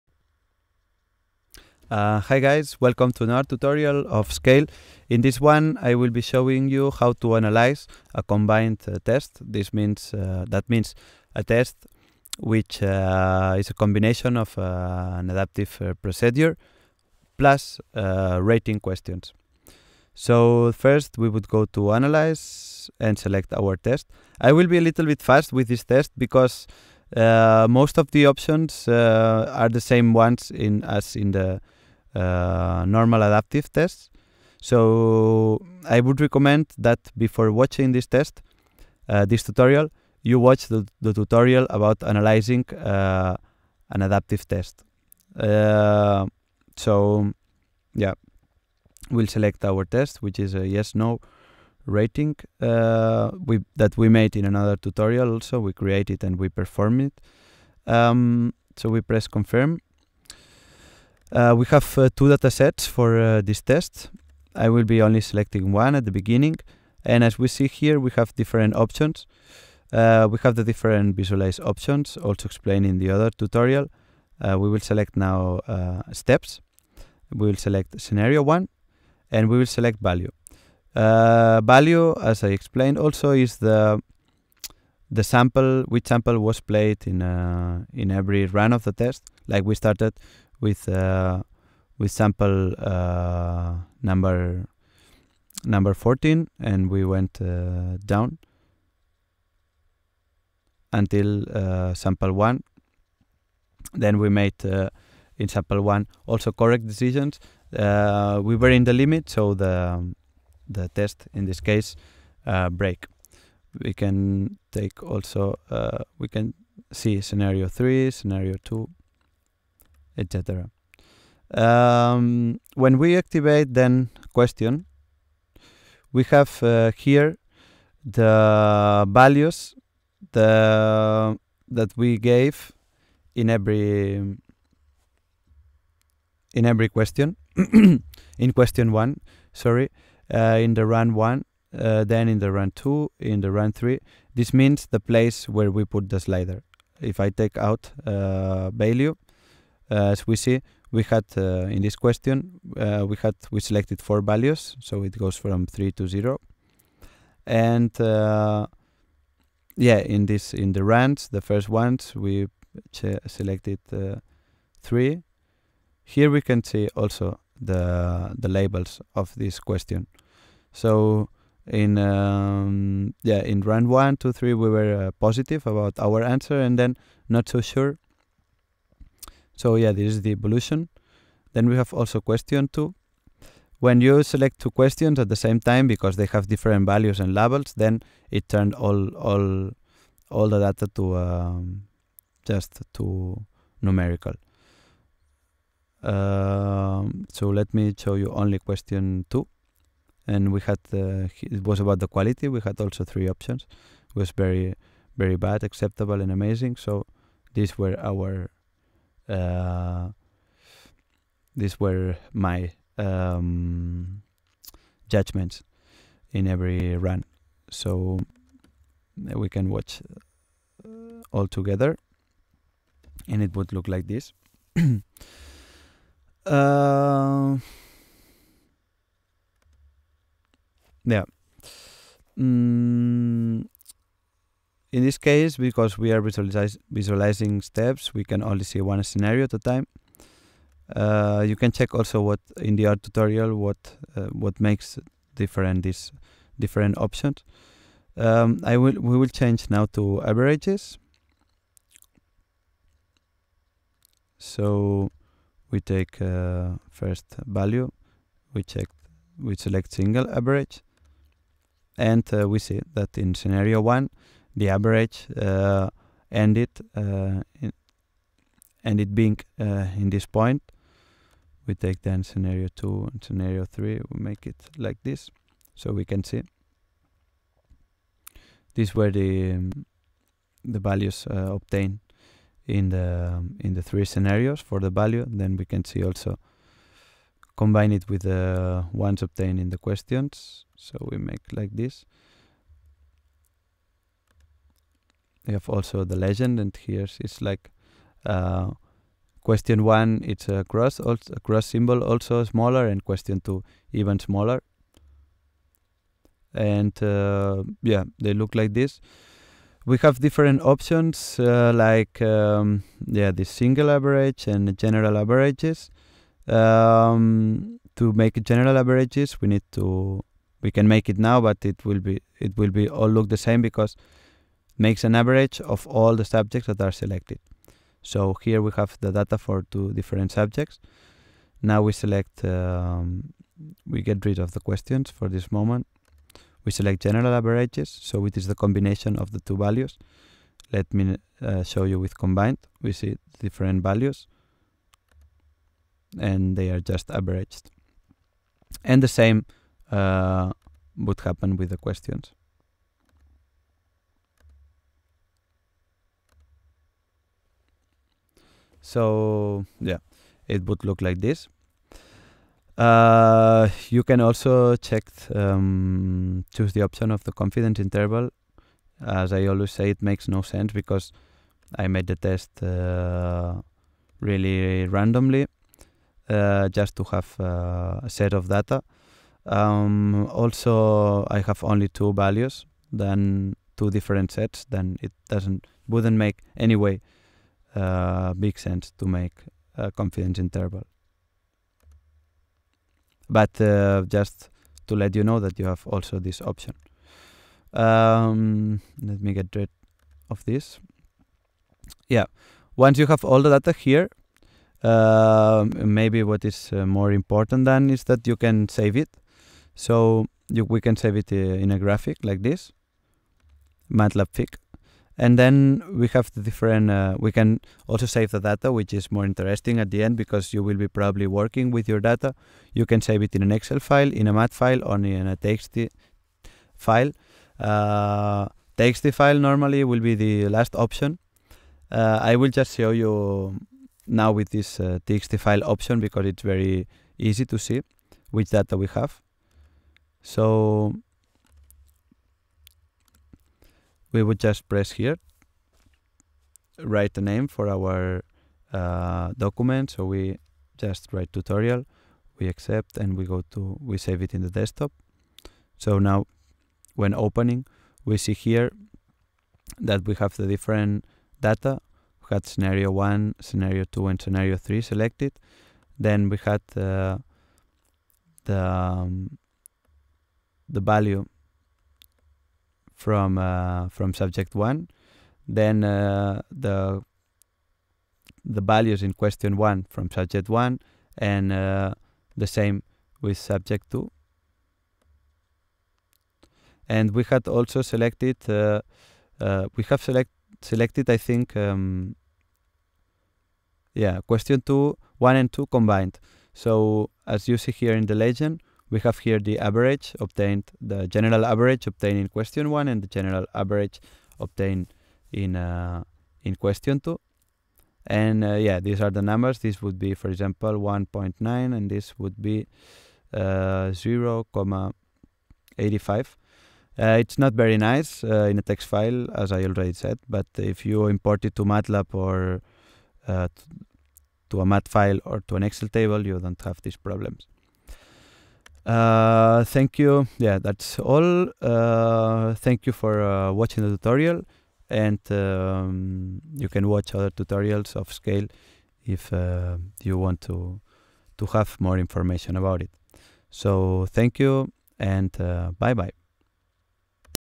Scale tutorial "Analysing a Yes/No plus Rating Questions Test"
In this tutorial the analysis of a Yes/No test plus Rating Questions using the software for Listening Experiments Scale is explained.